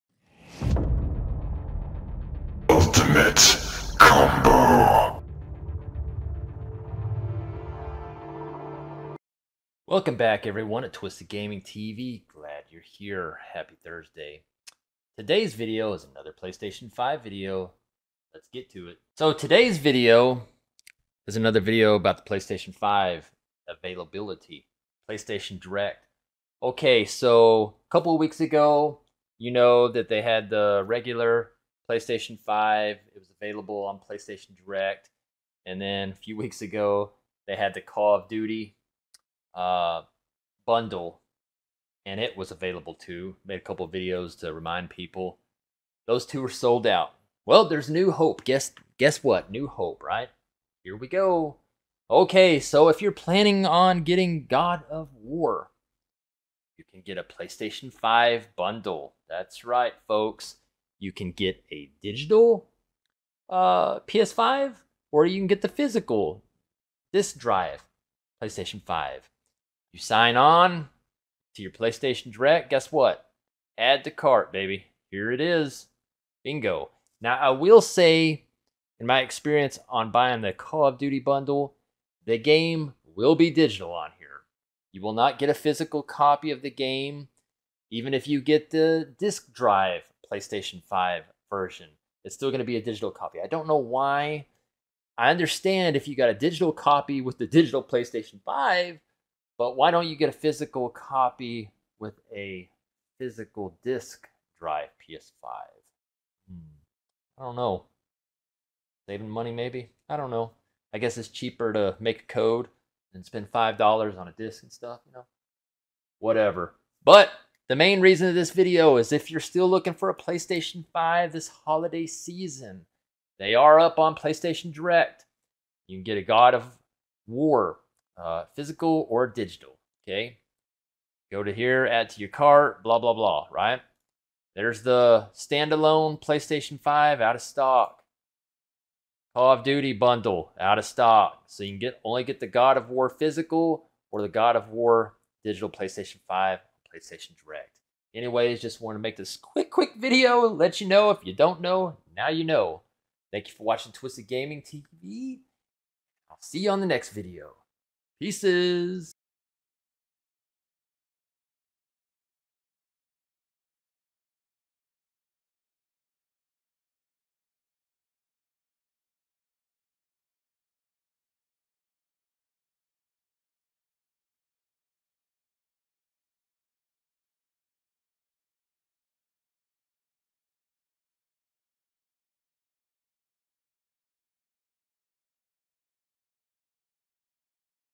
PlayStation 5 God of War Ragnarök Bundle Preorder Available!
Welcome back to the channel.  Today's video is another PlayStation 5 video talking about the PS5 God of War Ragnarok bundle!  There is PS5's available for preorder on PlayStation direct right now if you need one!